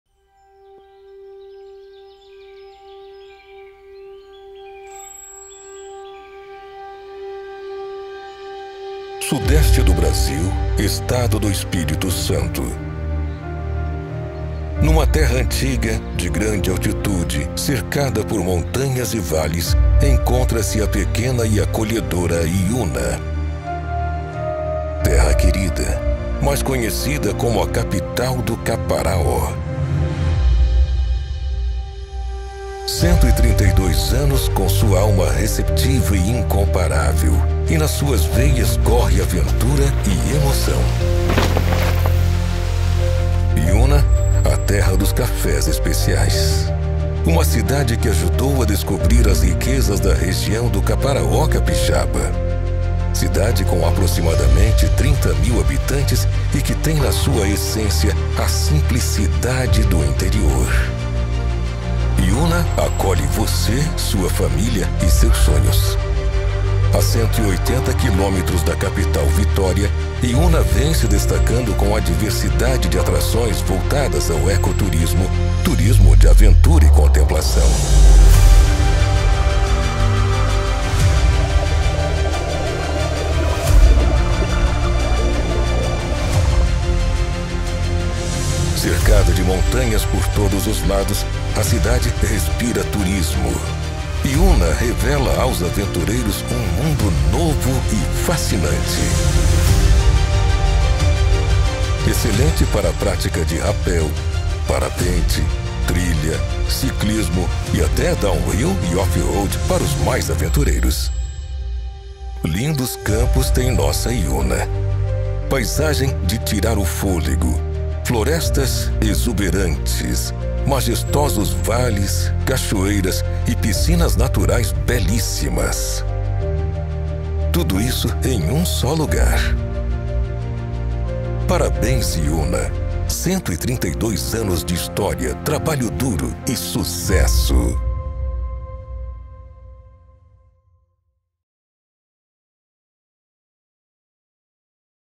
Iúna, 132 anos de emancipação, história, trabalho duro e sucesso
O município atualmente é administrado pelo prefeito Romário Batista Vieira e tem como vice-prefeito Cláudio Deps.

Iúna tem consolidada a cultura cafeeira no cenário regional. Hoje somos o maior produtor de café arábica do Estado do Espírito Santo e o 9º (nono) maior produtor de café arábica do Brasil, segundo dados do IBGE 2020. Também somos destaque SIC (Semana Internacional do Café) em Belo Horizonte na produção de cafés especiais produzindo o 2º melhor café do Brasil em 2018, 8º melhor em 2019 e 3º melhor em 2020.

A cidade que ajudou a descobrir as riquezas da região do Caparaó Capixaba completa hoje, dia 24, 132 anos de emancipação. Com aproximadamente 30 mil habitantes tem na sua essência a simplicidade do interior.

Possui paisagens de tirar o fôlego, florestas exuberantes, majestosos vales, cachoeiras e piscinas naturais belíssimas.

Uma cidade com sua alma receptiva e incomparável onde nas suas veias corre aventura e emoção.

O prefeito Romário Batista Vieira parabeniza o município, “Iúna é uma cidade pequena, mas de grande coração. Temos aqui um povo acolhedor, produtores de ótimos produtos, empreendedores fantásticos, cada morador têm sua contribuição para que nossa querida Iúna seja tão especial. Nosso município cresceu muito, estamos sendo reconhecidos com nossos cafés em todo o Brasil. Parabéns, minha querida Iúna!”.

Parabéns Iúna. 132 anos de história, trabalho duro e sucesso.